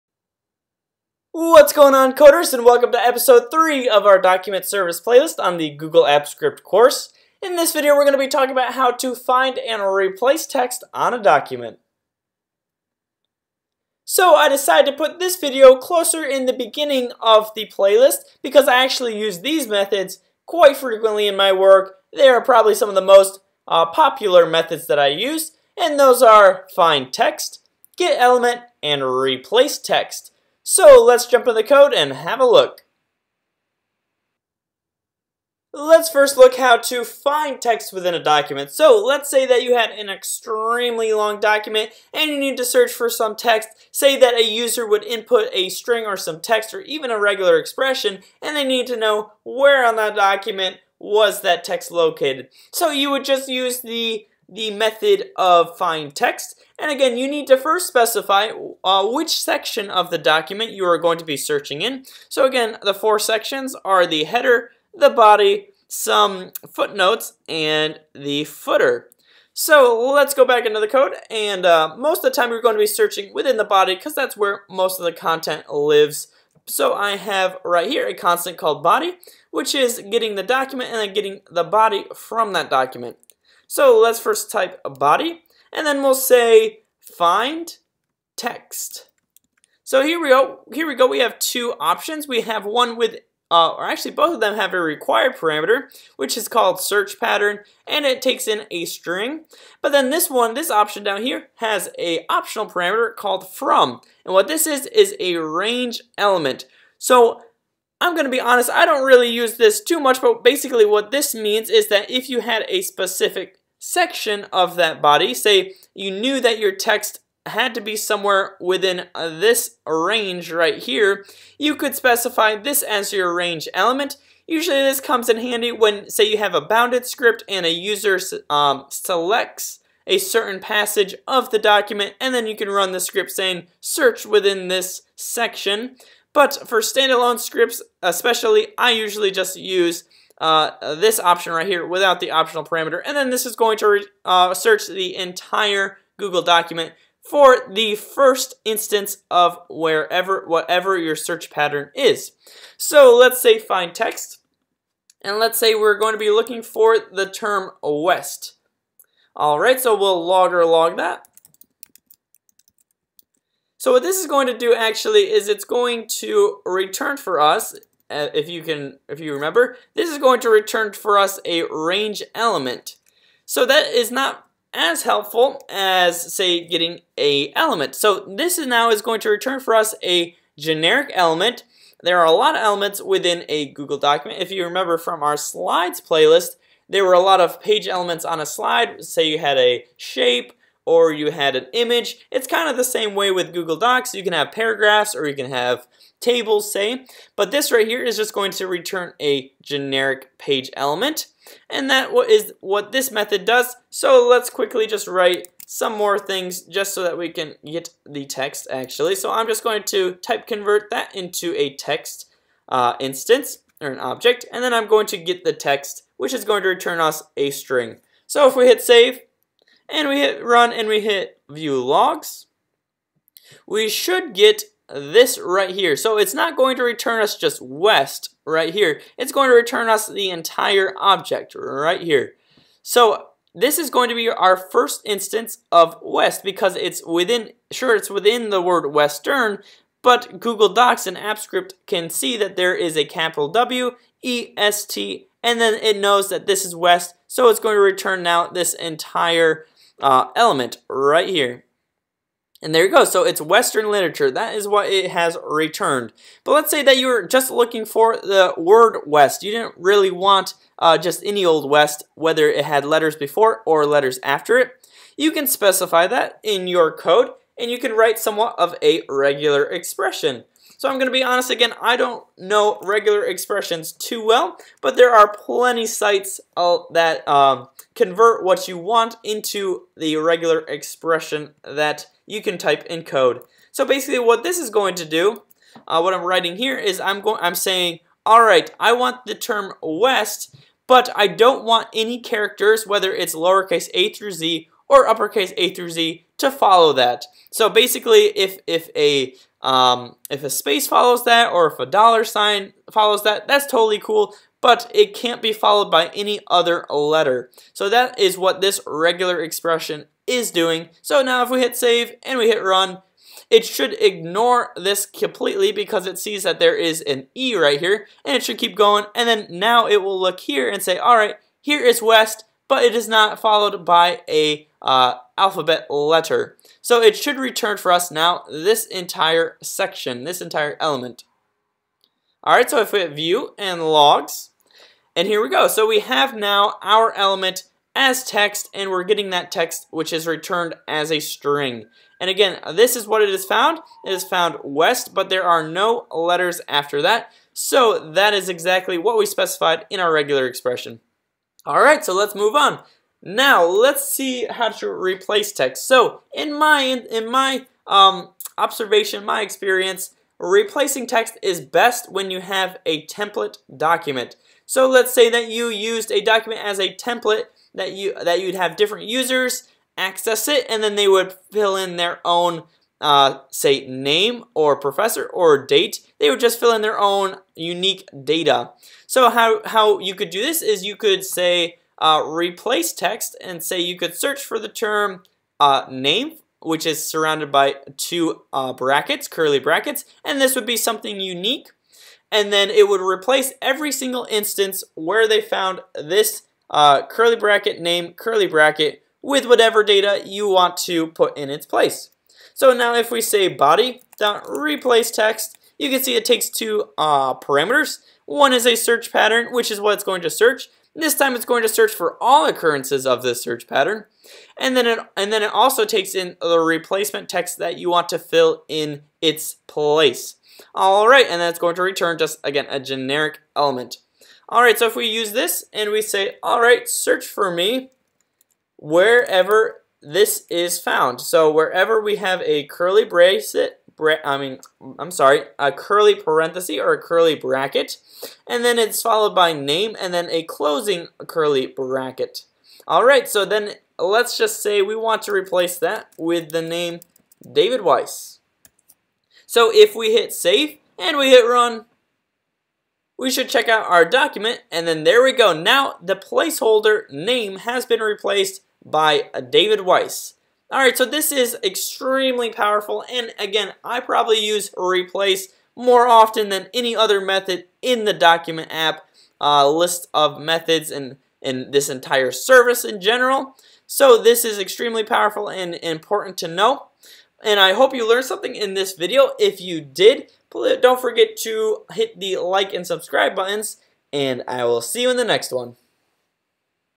Find & Replace Text - Episode 6.3 | Apps Script ~ Document Service
Hey Coders! Often, documents will need referencing or modification. One of the biggest use cases of DocumentApp involves creating a Google Document that acts as a template, and then replacing the placeholders with actual user data. In order to know how this can be done, you'll first have to learn how to find and replace text, showcased in this video!